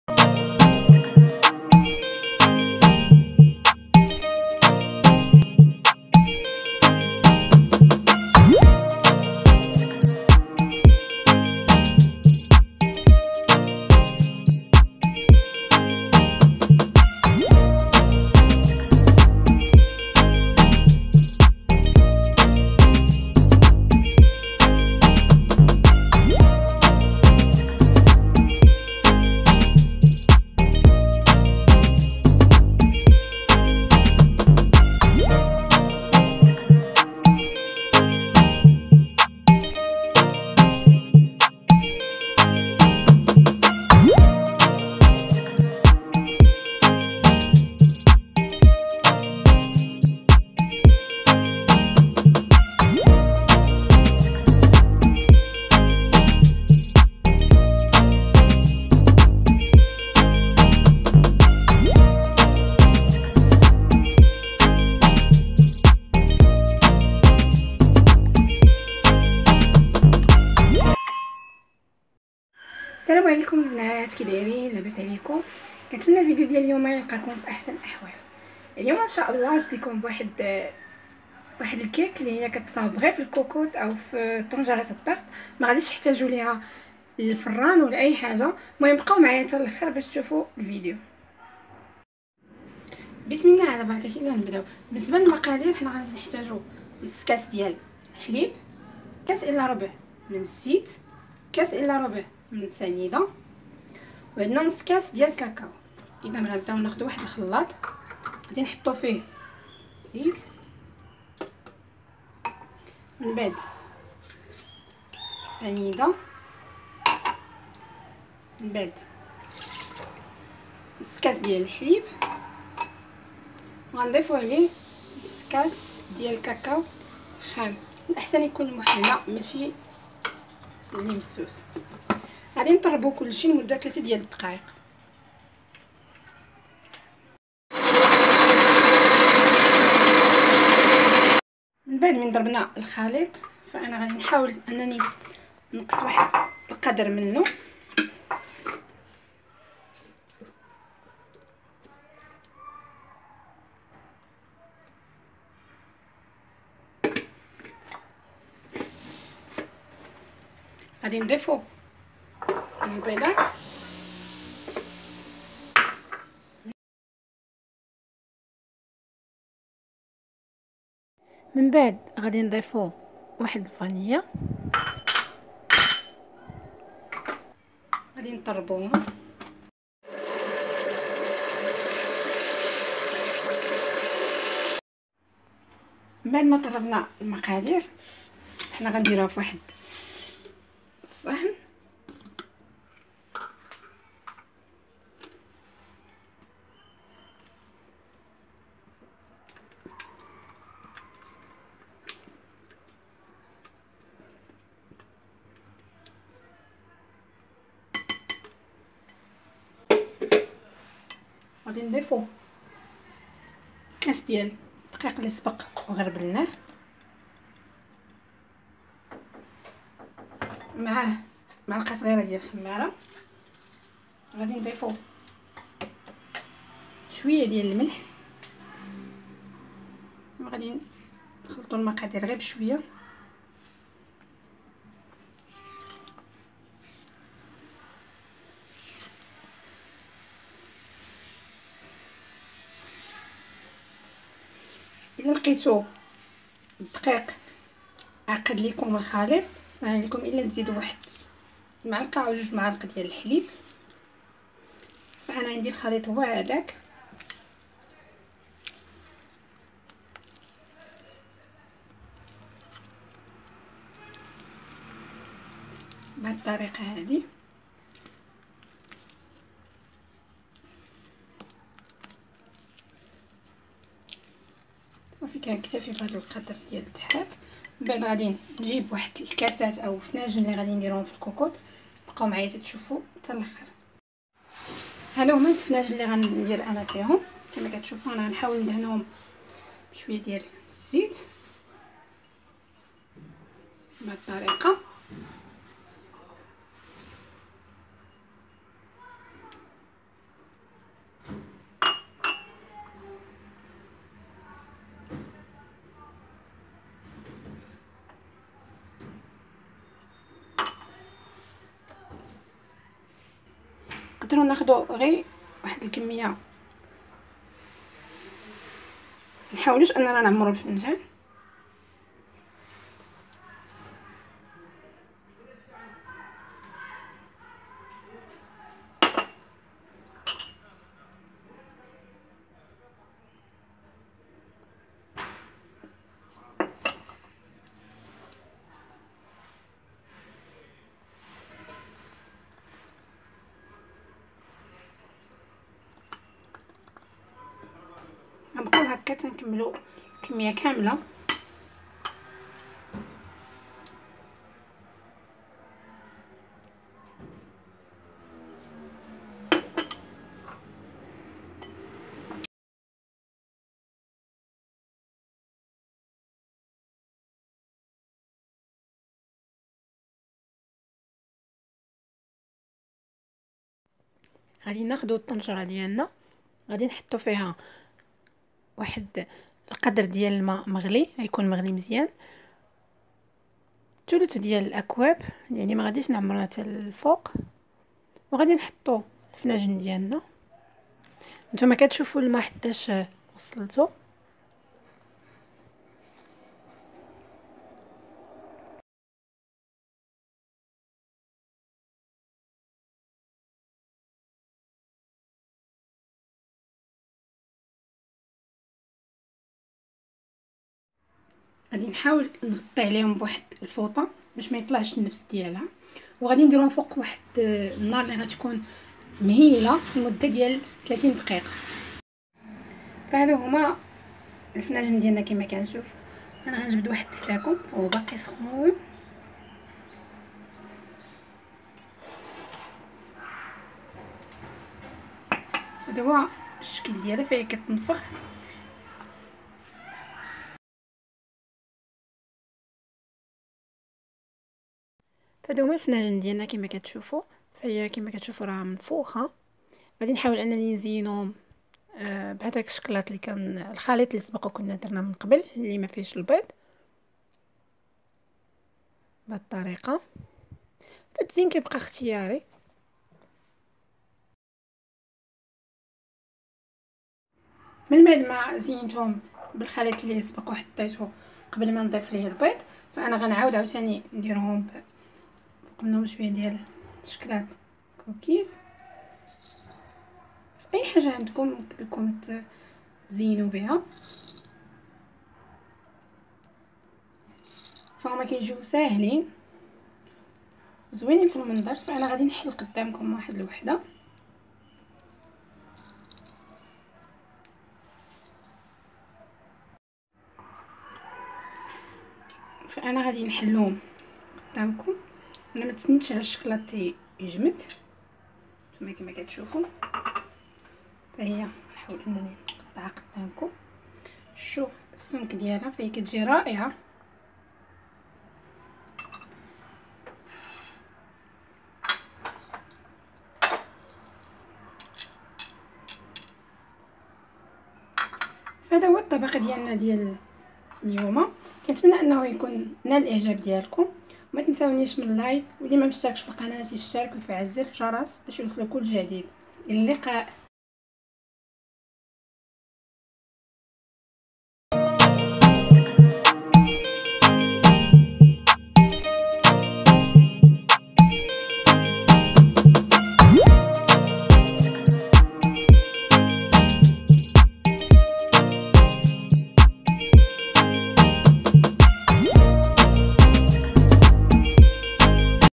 مافن او كاب كيك الفنجان بلا فران بلا ميكرويف ساهلة وبسيطة في وقت وجيز
تفعيل زر الجرس🔔 و اللايك
      ❤فضلا و ليس امرا ❤